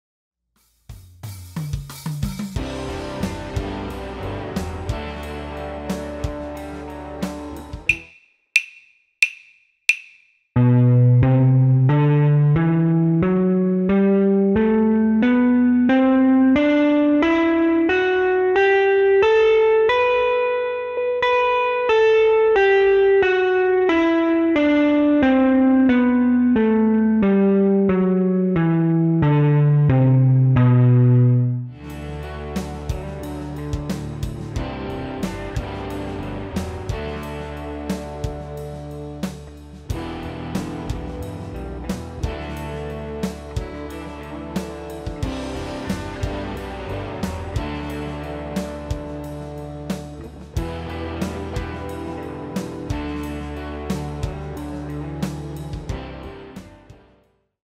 Phrygian for guitar
This video teaches Phrygian in the key of G major.

Here is a link to a Play Along Loop in the key of G. You can use this video as a backing track while you practice the scales you are learning.